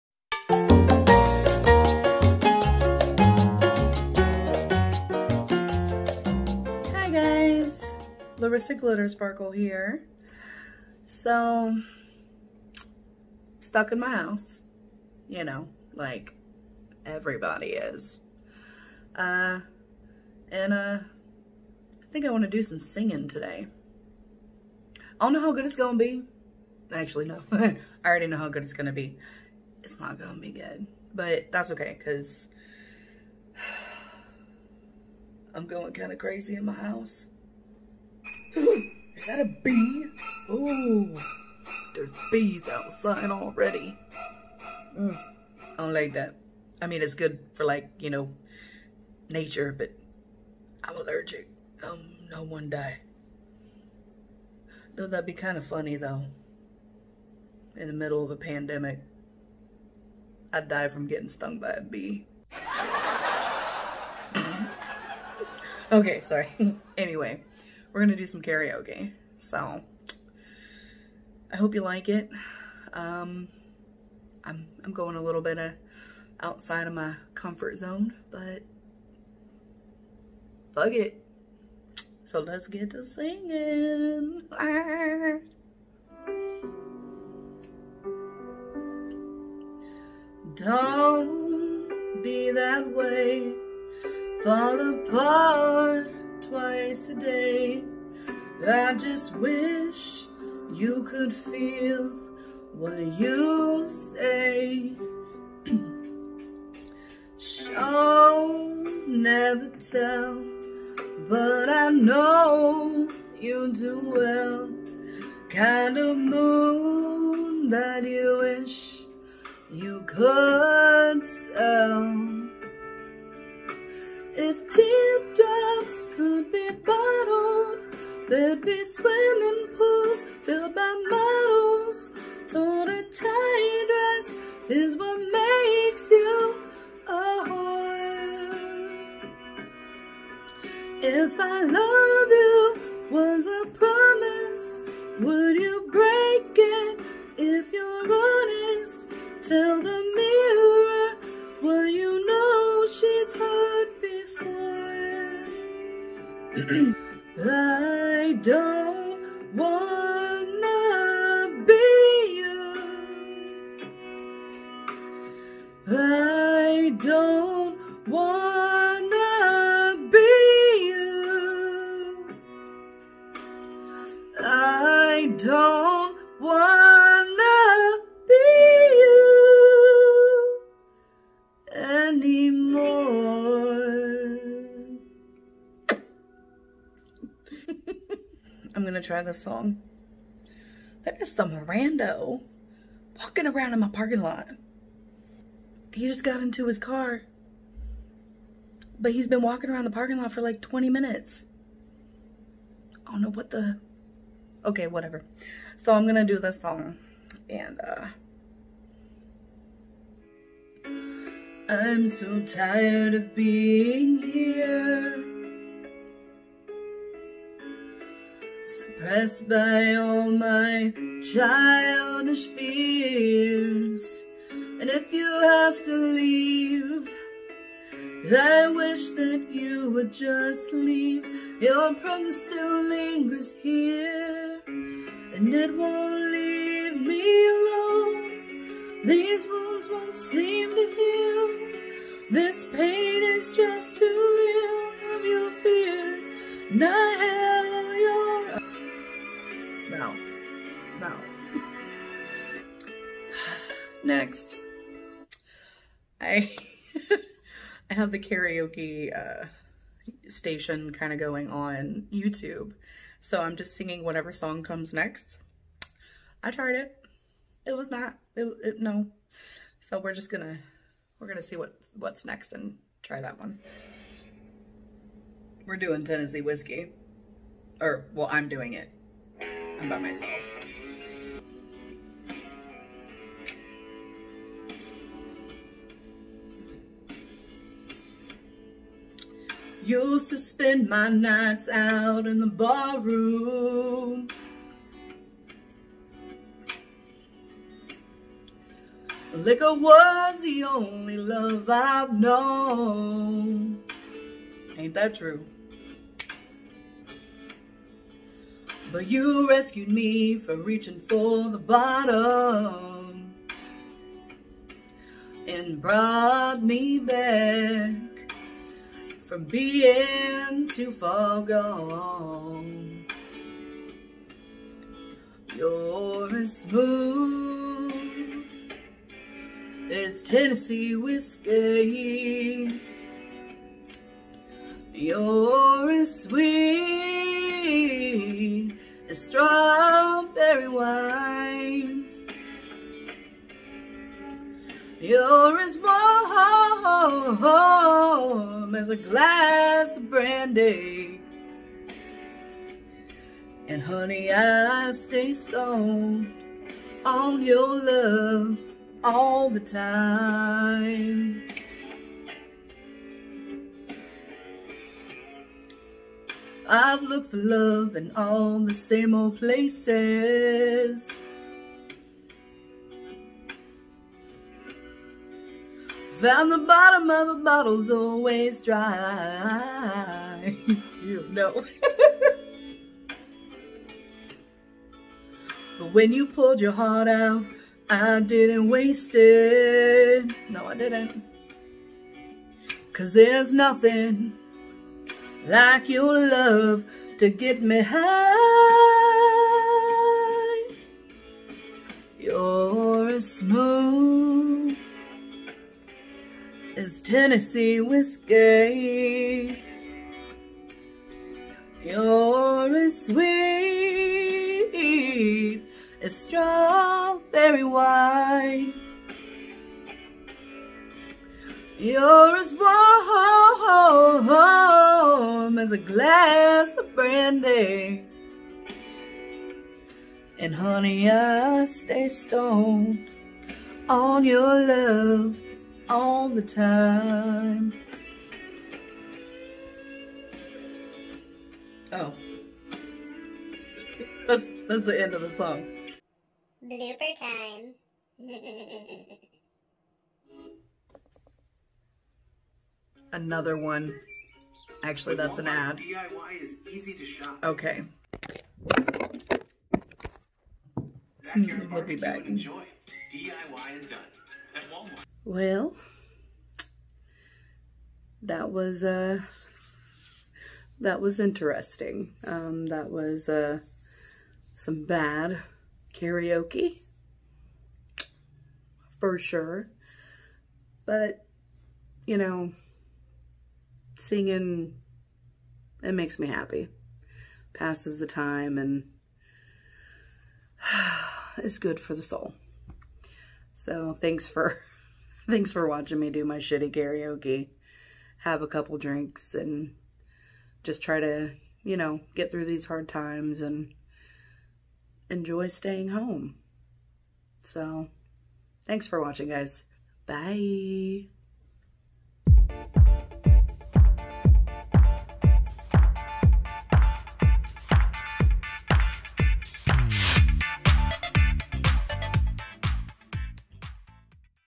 Quarantine Karaoke 2020
Just singing my heart out and trying to make it through these rough times.